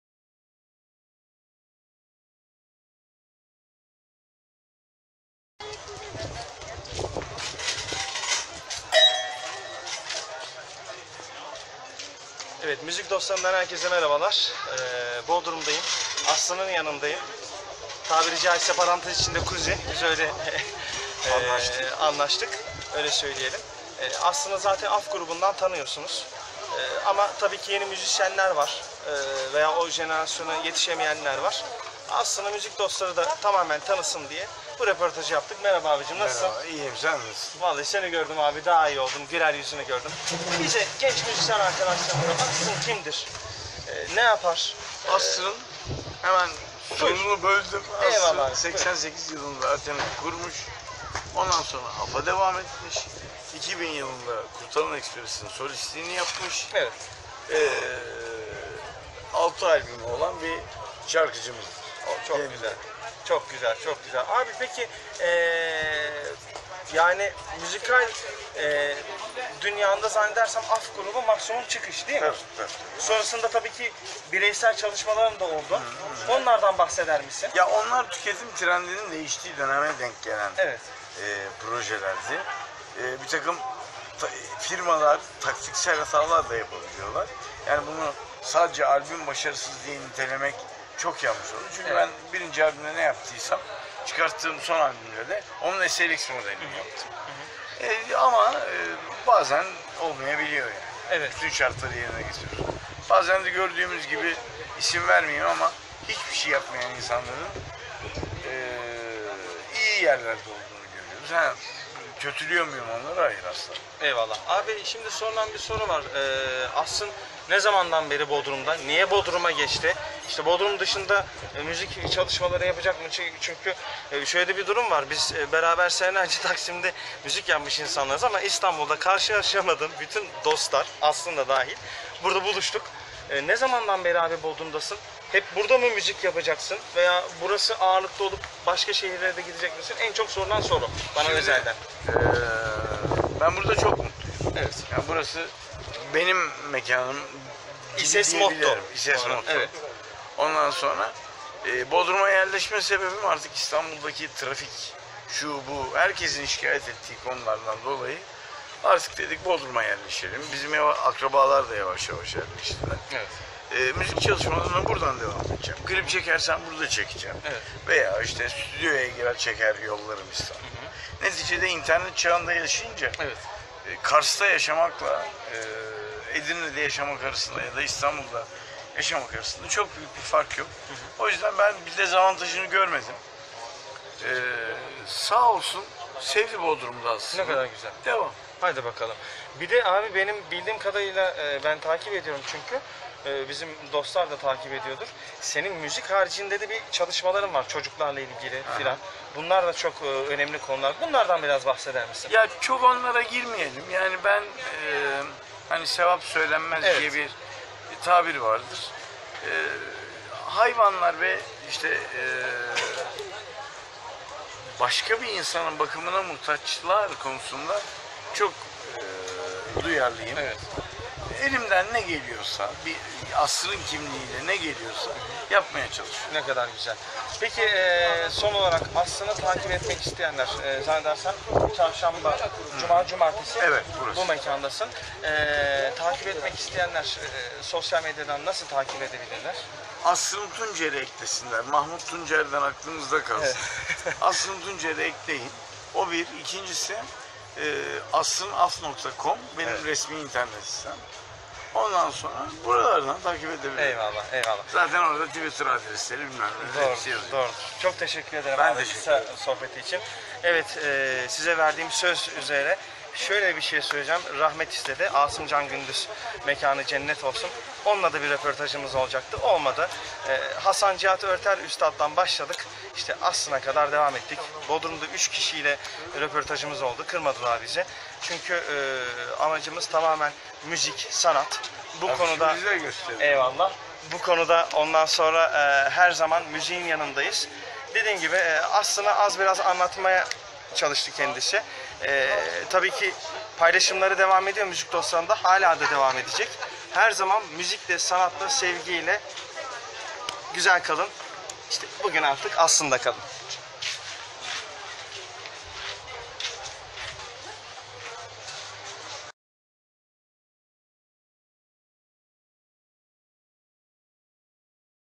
KENAN DAMAR & ASRIN TUNCER (AF) - MÜZİK DOSTLARI BULUŞMASI
Ve AF grubunun kurucusu emektar müzisyen Asrın Tuncer ile Bodrum'da sıcak ve samimi sohbet tadında röportajımız oldu. Buyurun MüzikDostları iyi seyirler...

Yeni projelerimiz için bizi takip edin;)